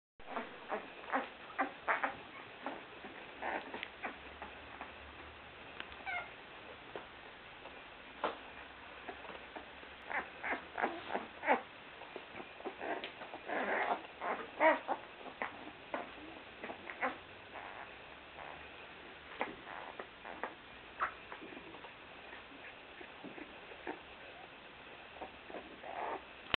Male Pups at nearly 2 weeks old
Here are the males in the nest of Pandora at nearly 2 weeks of age.